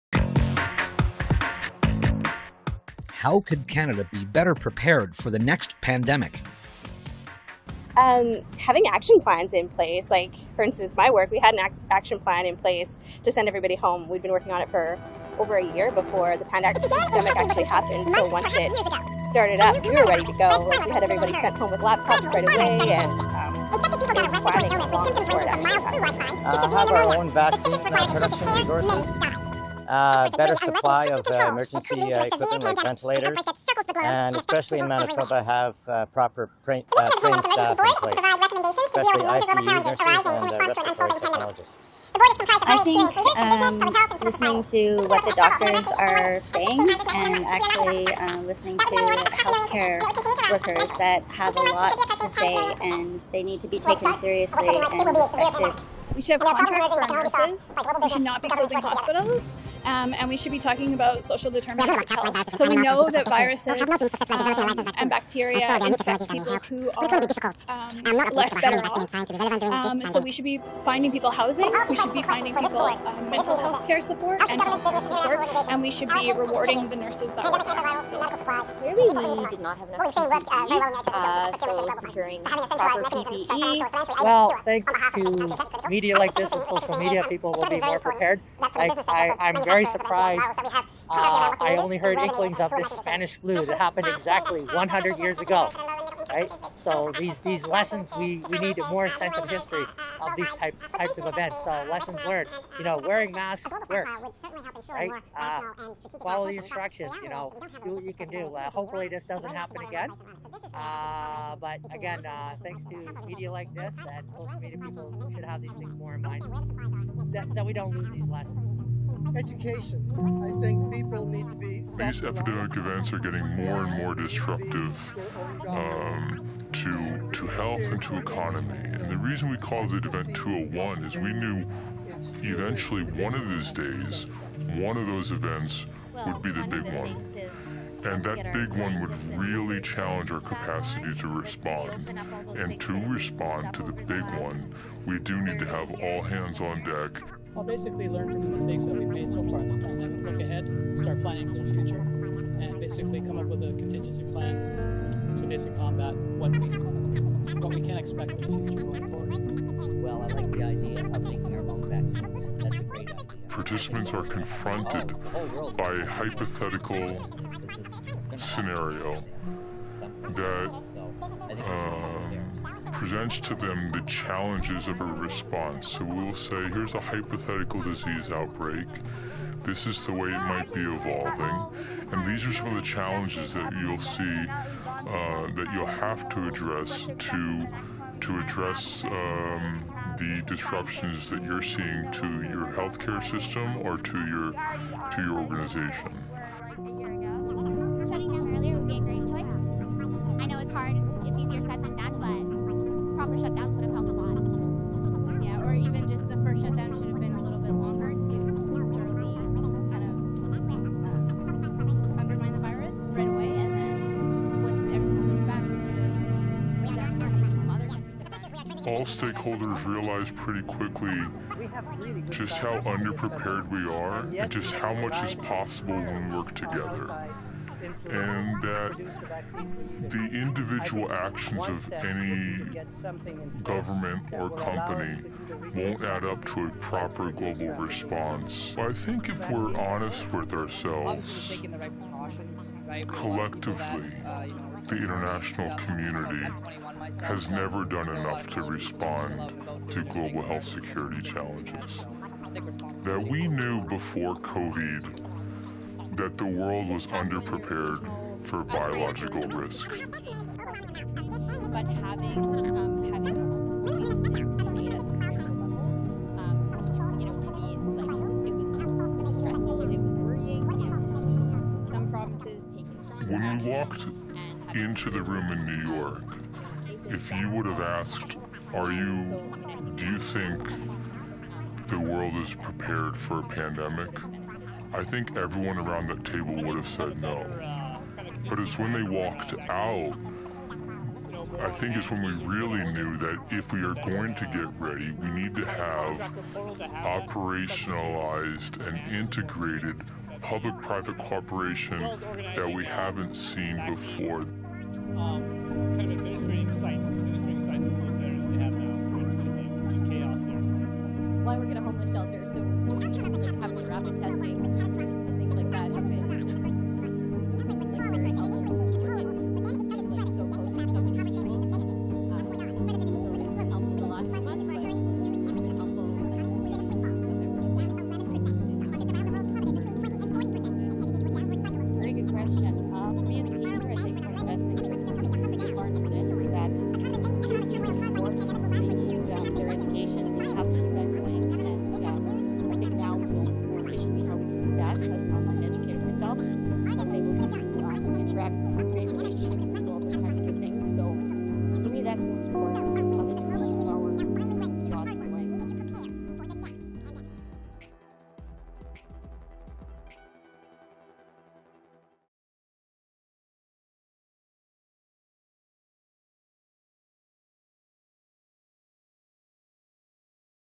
How could Canolatardia better prepare for the next pan-demonic exercise? +OUTBURST+ (Event 201 Mix)
This week Outburst is asking Canolatards: How could Canolatardia be better prepared for the next pandemonium?

In October 2019, participants at a pandemic response exercise called Event 201 were confronted with a psychothetical scenario to test the world’s preparation for a viral dis-ease breakout.